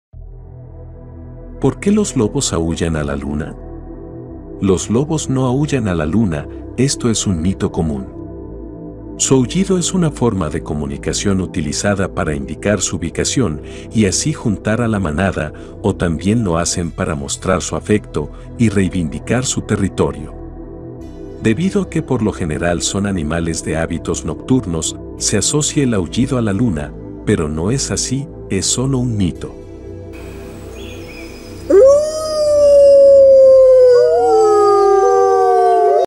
¿Por qué los lobos aúllan a la luna?
La Luna se enamoró del espíritu del lobo, y en un ataque de egoísmo se llevó su sombra para recordar para siempre esa noche. Desde entonces el lobo aúlla a la Luna pidiéndole por favor que le devuelva su sombra.